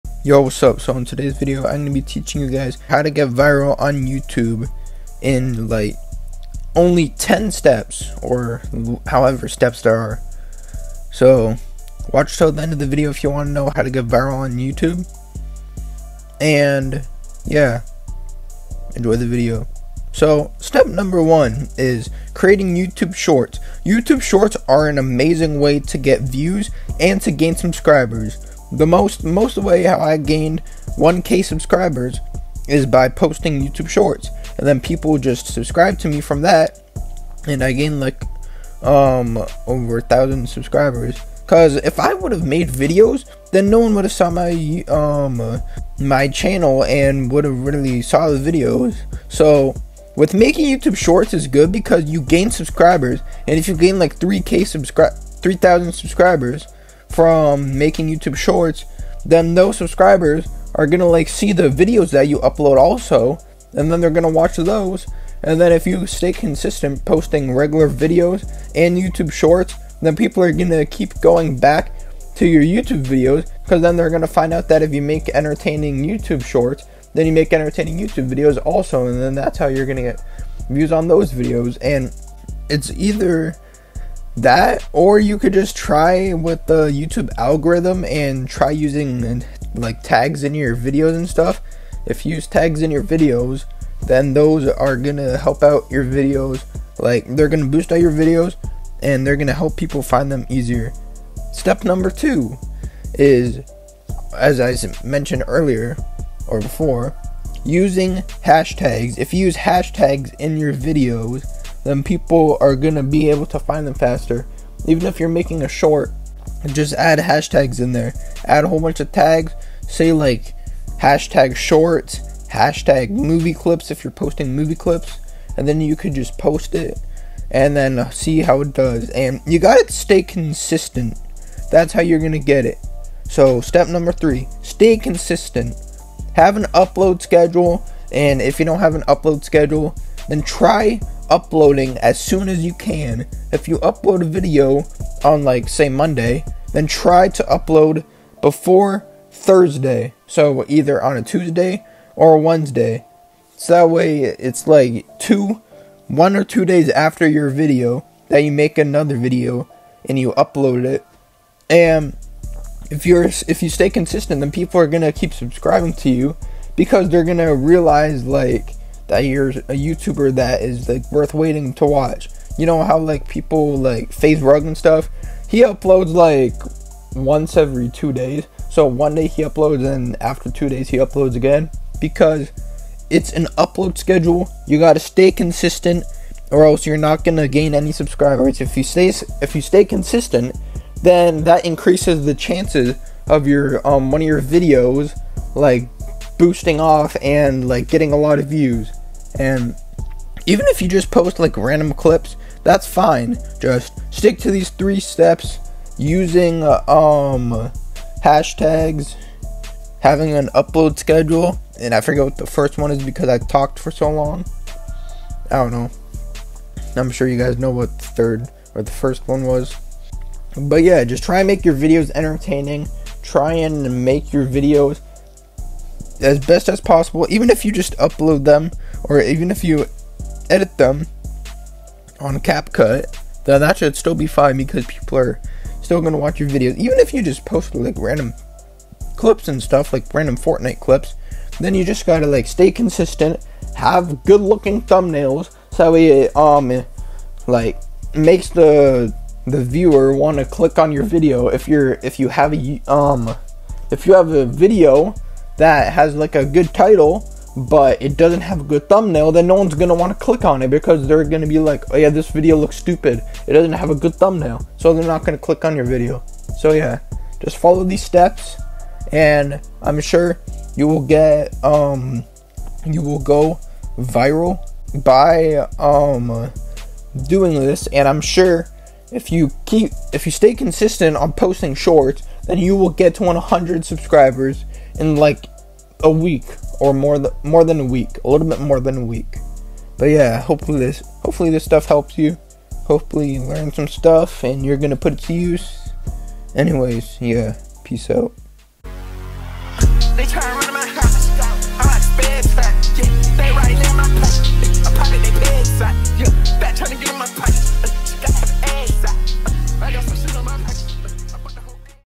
How to get 1000 Subscribers In A Week
These are some tips on how to gain 1000 subs on youtube in a week